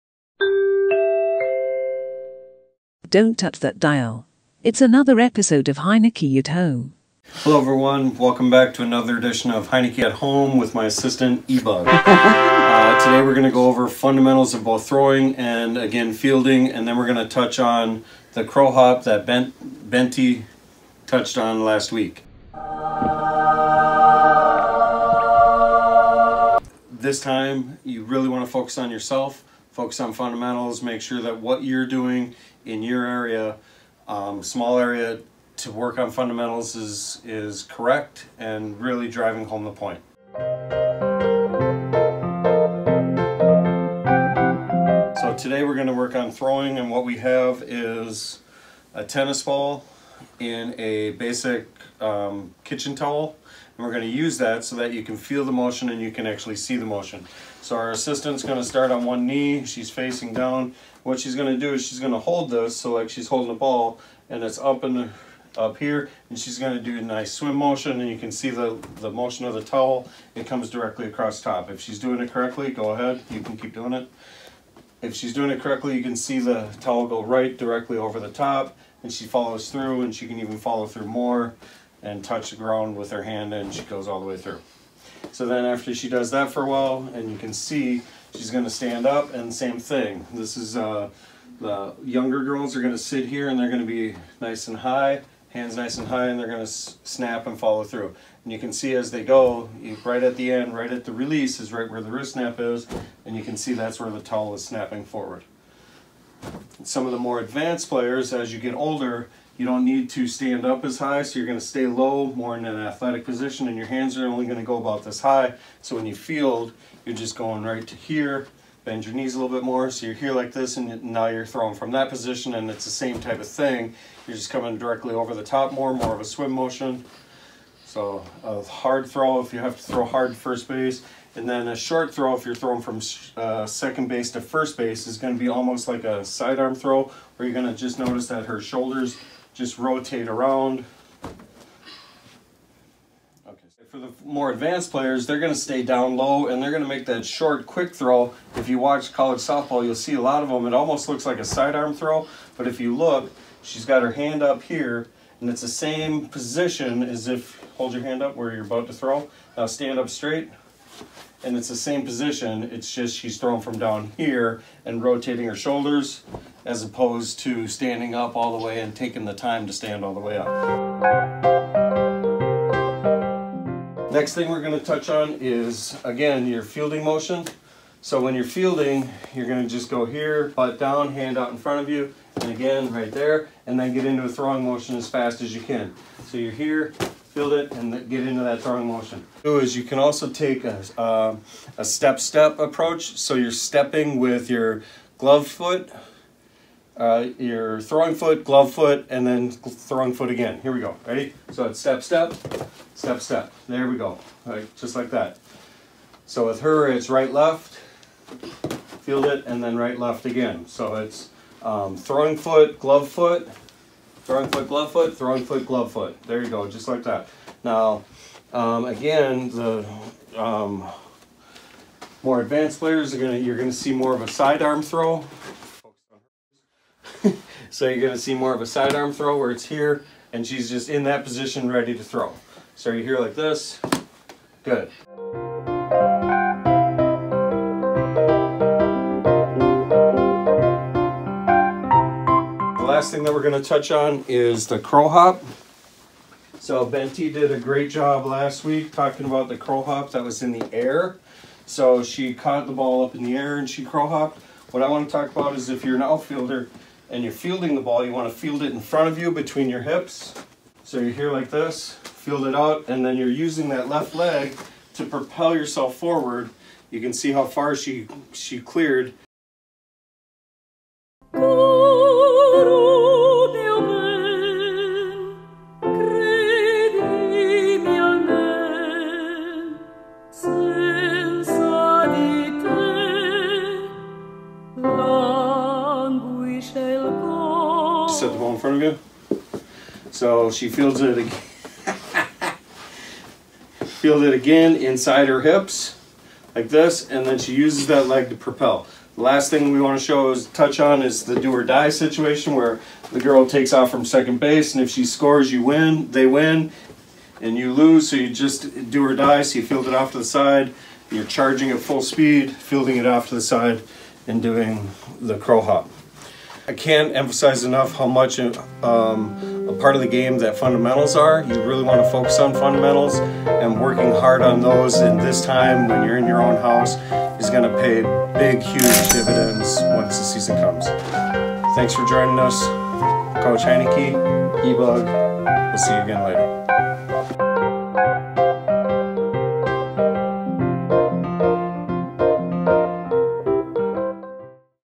Heinecke at Home: Coach Tom continues his series with throwing, fielding, footwork + crow hop pt. 2
612 Fastpitch Vice-President Tom Heinecke is back again with another virtual lesson. He starts of with a tennis ball towel throwing drill, transitions to footwork and fielding and finished with a crow hop for balls fielded on the ground including do or die throws. Work hard. Be safe. You can count on 6-1-2.